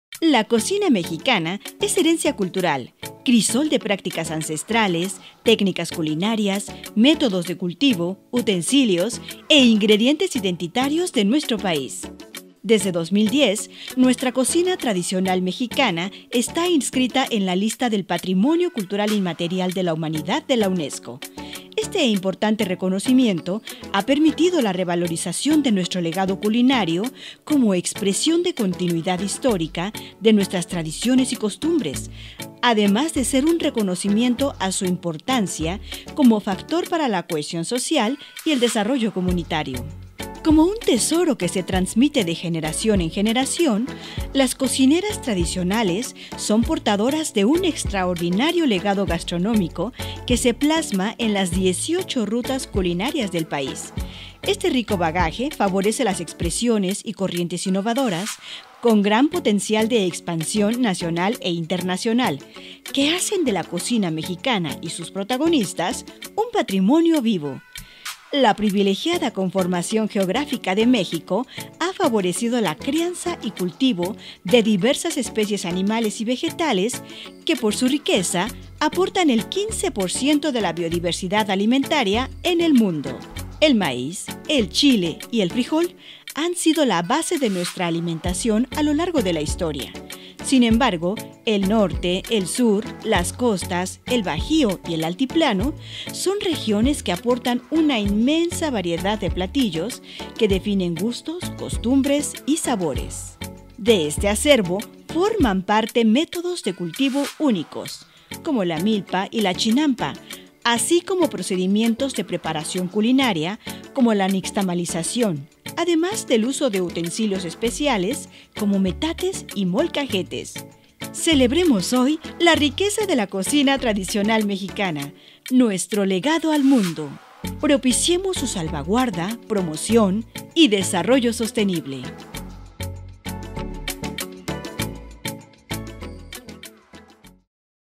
Día Nacional de la Gastronomía Mexicana
La cocina tradicional mexicana es herencia cultural 🌶🌽🥑🌮🍮🥗 Desde el 2010 está inscrita en la lista del Patrimonio Cultural Inmaterial de la Humanidad de la UNESCO, permitiendo la revalorización de nuestro legado culinario.
En CulturaUDG celebramos hoy, 16 de noviembre, el DíaNacionaldelaGastronomíaMexicana. ¿Cuál es tu platillo favorito?